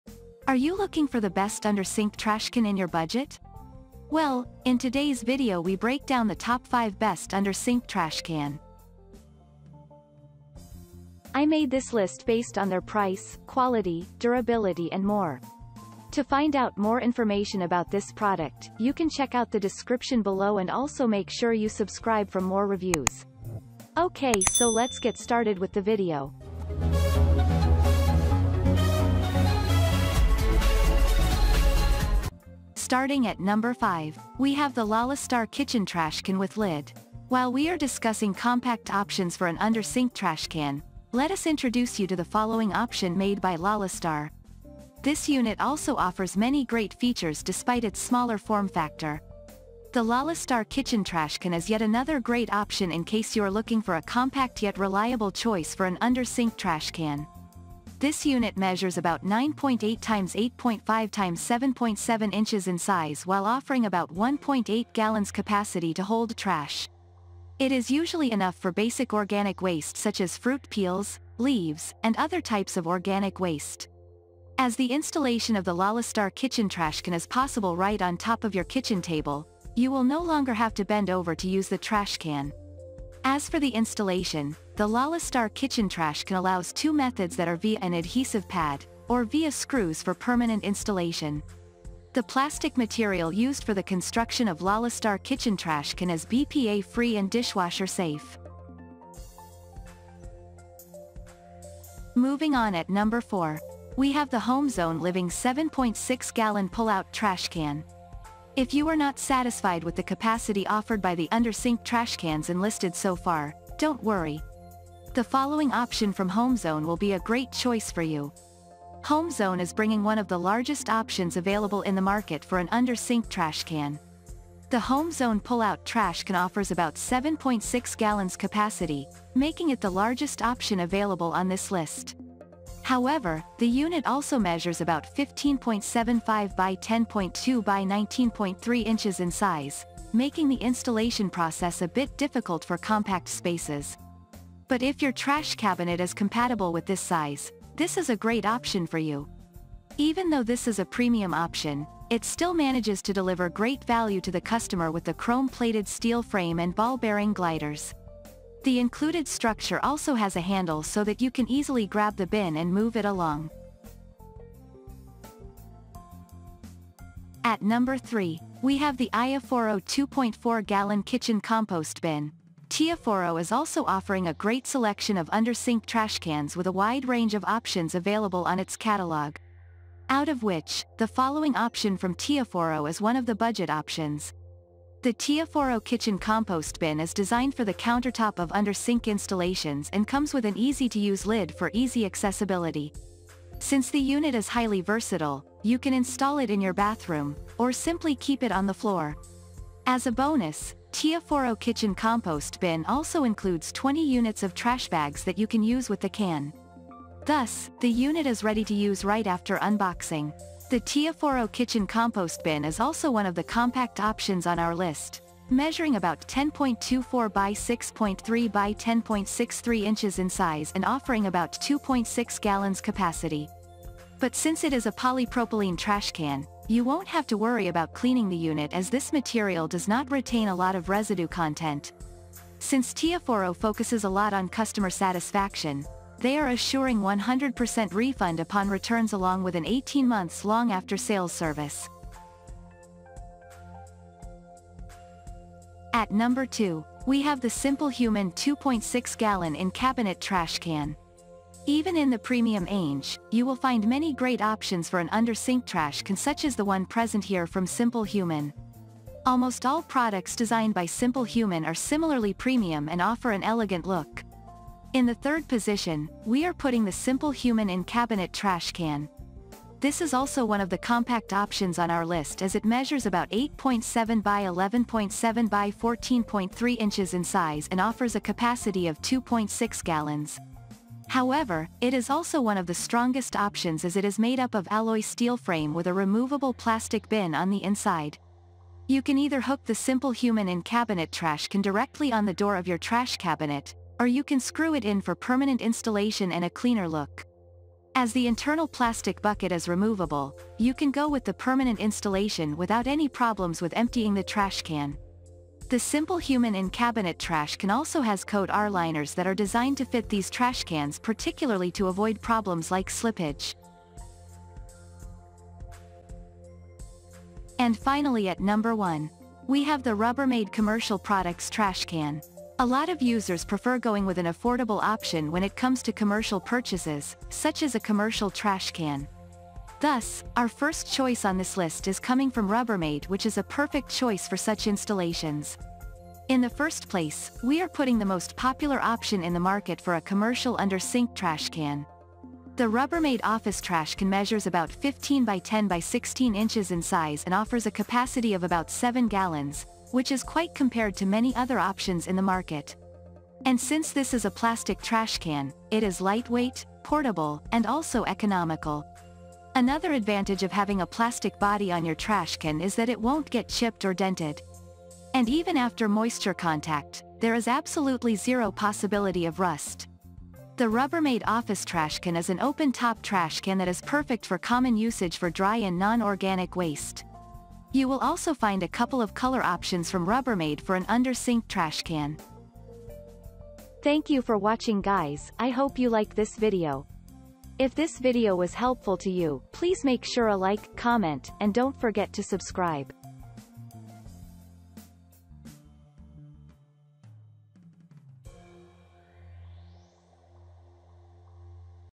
5 Best Under Sink Trash Can for 2023
▶️ In This video We recommended the top 5 best Under Sink Trash Can for 2023.

▶️ 5. Lalastar Kitchen Trash Can with Lid.

▶️ 4. Home Zone Living 7.6 Gallon Pull Out Trash Can.

▶️ 3. iyafuro 2.4 Gallon Kitchen Compost Bin.

▶️ 2. Simplehuman 2.6 Gallon In-Cabinet Trash Can.

▶️ 1. Rubbermaid Commercial Products Trash Can.

In this video, we shortlisted the top 5 Best Under Sink Trash Cans on the market for 2023. We made this list based on our personal opinion. After performing our research based on their price, quality, durability, brand reputation, and other related issues, we ranked them with no particular request or influence.  All of them maintain quality. If you want to buy an Under Sink Trash Can, we think this list will be beneficial to you.

We hope you like the Under Sink Trash Can we selected for this year. If you have any questions related to the Under Sink Trash Can which are listed here, please leave a comment down below and we will get back to you as soon as possible.